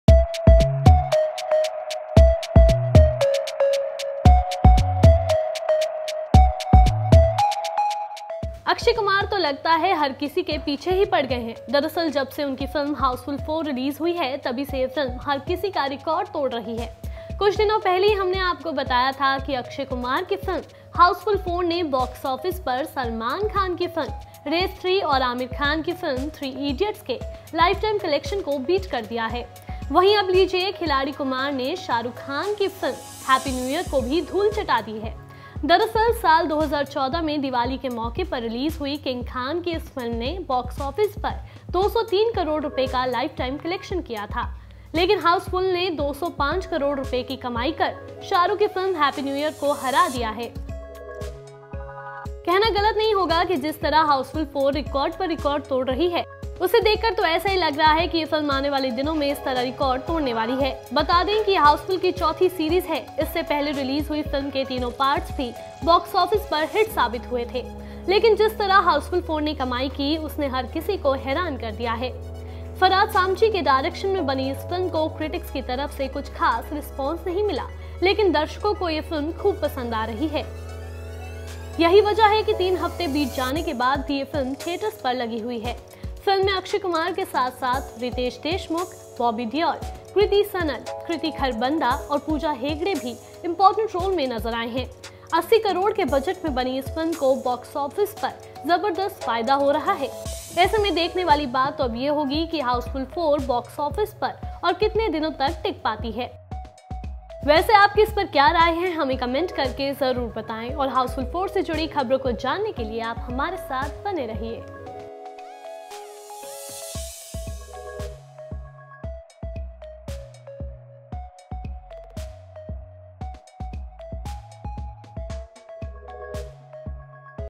Akshay की Housefull 4 ने Shahrukh का ताोड़ा Record, देखिए ये रिपोर्ट
India News